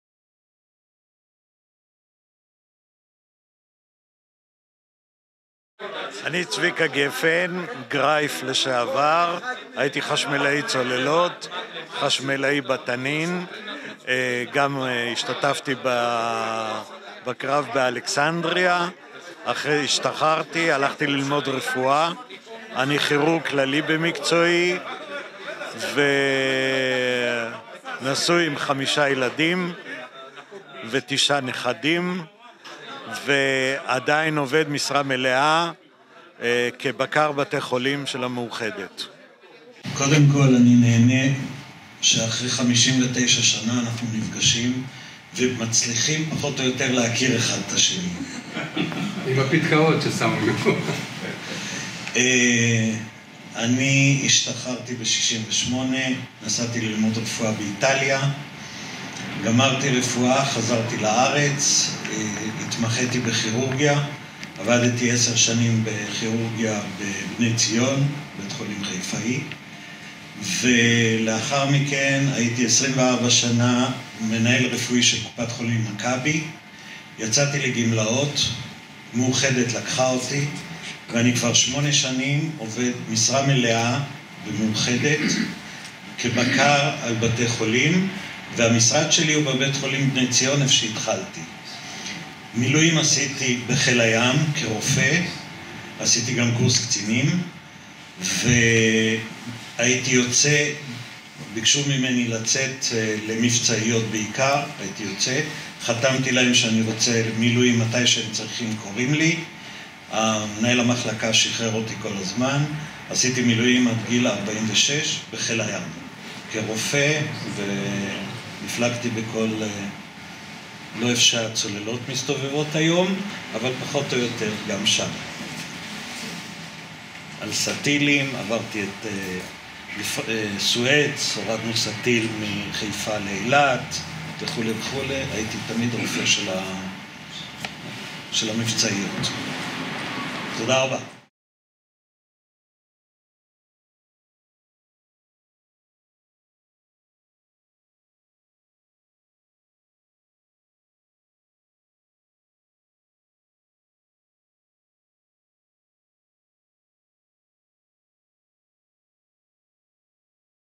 צבי גפן - שייטת הצוללות מחזור ח'.
בוגרי מחזור ח' שייטת הצוללות נפגשים אחרי 59 שנים. אפריל 2023.